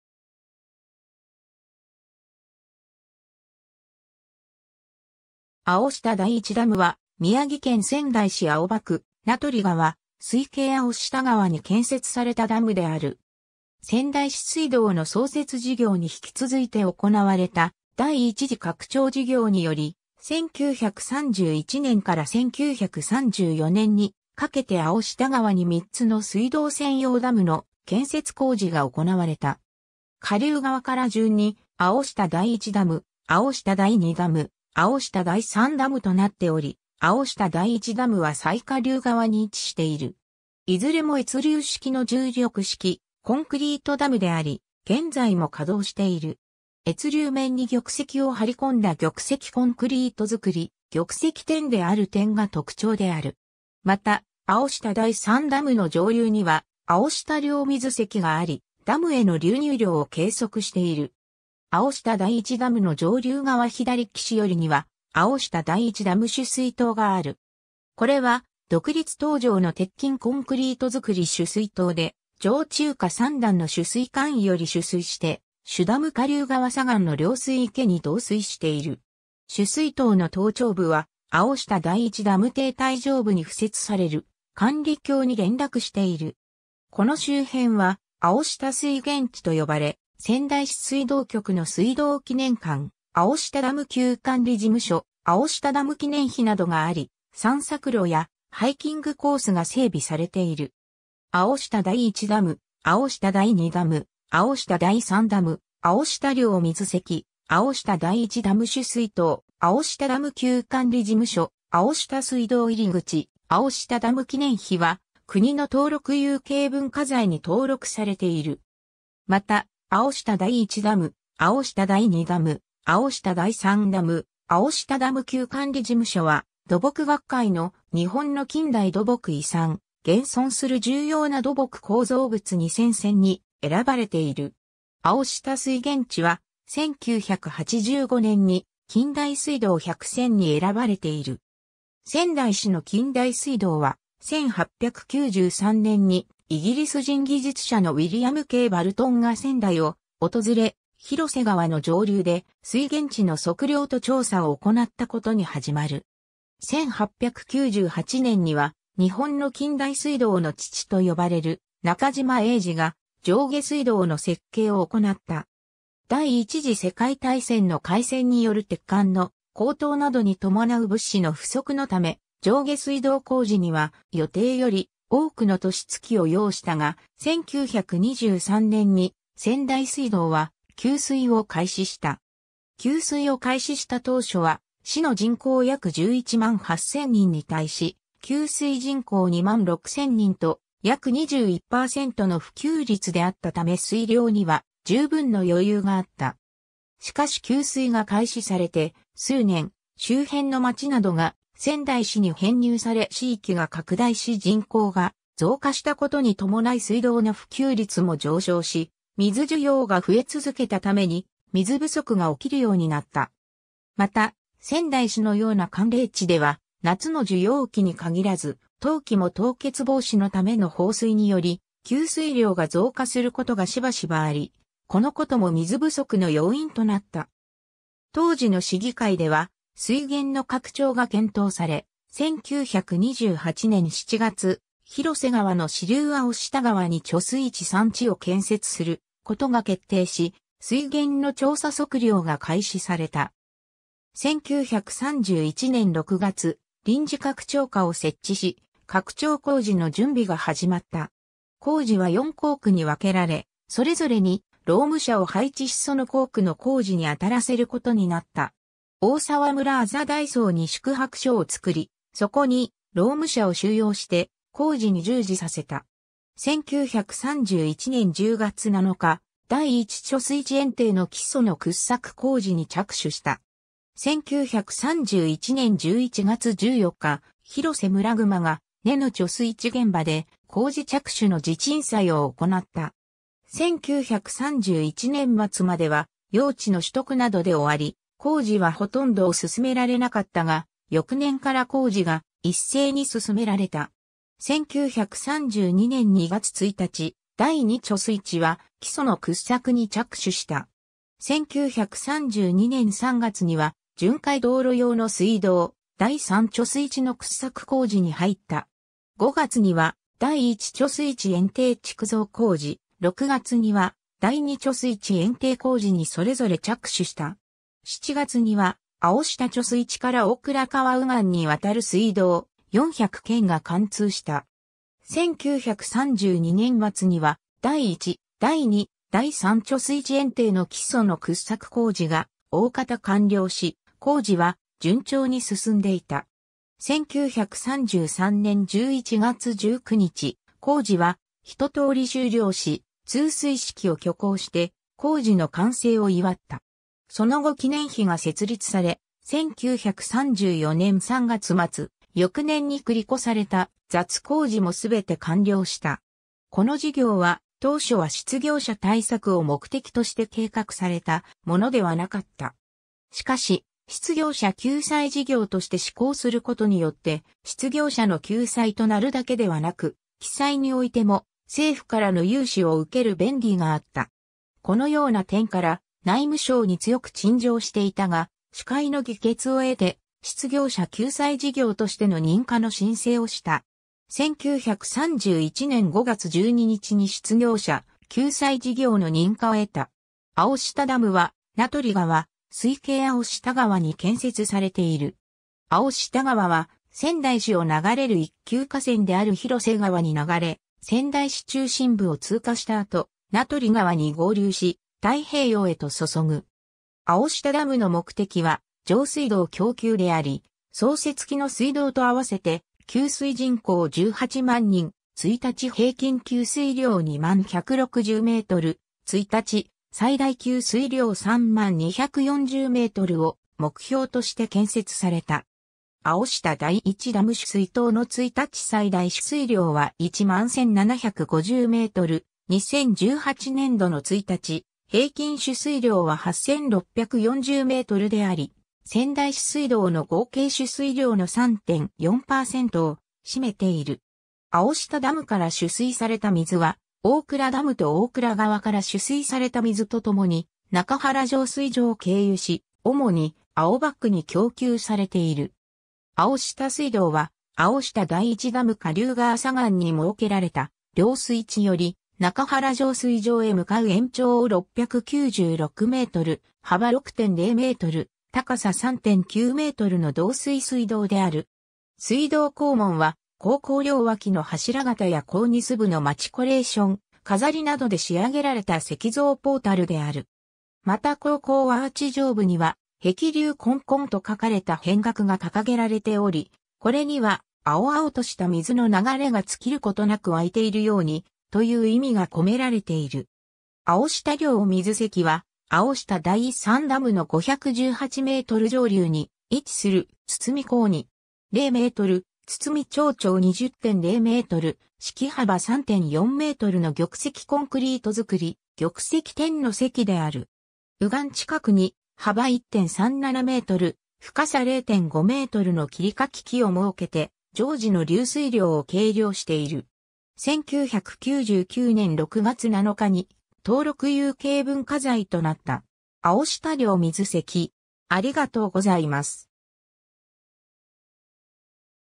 青下第1ダム
青下第1ダム（あおしただいいちダム）は宮城県仙台市青葉区、名取川水系青下川に建設されたダムである。
 仙台市水道の創設事業に引き続いて行われた第一次拡張事業により、1931年（昭和6年）から1934年（昭和9年）にかけて青下川に三つの水道専用ダムの建設工事が行われた。
下流側から順に青下第1ダム、青下第2ダム、青下第3ダムとなっており、青下第1ダムは最下流側に位置している。
いずれも越流式の重力式コンクリートダムであり，現在も稼働している。
越流面に玉石を貼り込んだ玉石コンクリート造玉石貼である点が特徴である。
また、青下第3ダムの上流には青下量水堰があり、ダムへの流入量を計測している。
 青下第1ダムの上流側左岸寄りには青下第1ダム取水塔がある。
これは独立塔状の鉄筋コンクリート造取水塔で、上中下三段の取水管より取水して主ダム下流側左岸の量水池に導水している。
取水塔の塔頂部は青下第1ダム堤体上部に付設される管理橋（青下橋）に連絡している。
 この周辺は青下水源地とよばれ、仙台市水道局の水道記念館、青下ダム旧管理事務所、青下ダム記念碑などがあり、散策路やハイキングコースが整備されている。
 青下第1ダム、青下第2ダム、青下第3ダム、青下量水堰、青下第1ダム取水塔、青下ダム旧管理事務所、青下隧道入口、青下ダム記念碑は、国の登録有形文化財に登録されている。
また、青下第1ダム、青下第2ダム、青下第3ダム、青下ダム旧管理事務所は、土木学会の「日本の近代土木遺産―現存する重要な土木構造物2000選」に選ばれている。
青下水源地は、1985年（昭和60年）に近代水道百選に選ばれている。
 仙台市の近代水道は、1893年（明治26年）にイギリス人技術者のウィリアム・K・バルトンが仙台を訪れ、広瀬川の上流で水源地の測量と調査をおこなったことに始まる。
1898年（明治31年）には、日本の近代水道の父と呼ばれる中島鋭治が上下水道の設計をおこなった。
第一次世界大戦の開戦による鉄管の高騰などに伴う物資の不足のため上下水道工事には予定より多くの年月を要したが、1923年（大正12年）に仙台水道は給水を開始した。
給水を開始した当初は市の人口約11万8000人に対し給水人口2万6000人と約21%の普及率であったため水量には十分の余裕があった。
しかし給水が開始されて数年、周辺の町などが仙台市に編入され市域が拡大し人口が増加したことに伴い水道の普及率も上昇し、水需要が増え続けたために水不足が起きるようになった。
また、仙台市のような寒冷地では、夏の需要期に限らず、冬季も凍結防止のための放水により給水量が増加することがしばしばあり、このことも水不足の要因となった。
 当時の市議会では水源の拡張が検討され、1928年7月、広瀬川の支流青下川に貯水池3池を建設することが決定し、水源の調査測量が開始された。
1931年6月、臨時拡張課を設置し、拡張工事の準備が始まった。
工事は4工区に分けられ、それぞれに労務者を配置しその工区の工事にあたらせることになった。
大沢村字大倉に宿泊所をつくり、そこに労務者を収容して工事に従事させた。
 1931年10月7日、第一貯水池堰堤の基礎の掘削工事に着手した。
 1931年11月14日、広瀬村熊ヶ根の貯水池現場で工事着手の地鎮祭を行なった。
1931年末までは用地の取得などで終わり、工事はほとんど進められなかったが、翌年から工事が一斉に進められた。
1932年2月1日、第二貯水池は、基礎の掘削に着手した。
1932年3月には巡回道路用の隧道、第三貯水池の掘削工事に入った。
5月には第一貯水池堰堤築造工事、6月には第二貯水池堰堤工事にそれぞれ着手した。
7月には青下貯水池から大倉川右岸に亘る隧道 400 間（約727メートル）が貫通した。
1932年末には第一、第二、第三貯水池堰堤の基礎の掘削工事が大方完了し、工事は順調に進んでいた。
1933年11月19日、工事は一通り終了し通水式を挙行して工事の完成を祝った。
その後記念碑が設立され、1934年3月末、翌年に繰り越された雑工事もすべて完了した。
 この事業は、当初は失業者対策を目的として計画されたものではなかった。
しかし、失業者救済事業として施工することによって、失業者の救済となるだけではなく、起債においても政府からの融資を受ける便宜があった。
このような点から、内務省に強く陳情していたが、...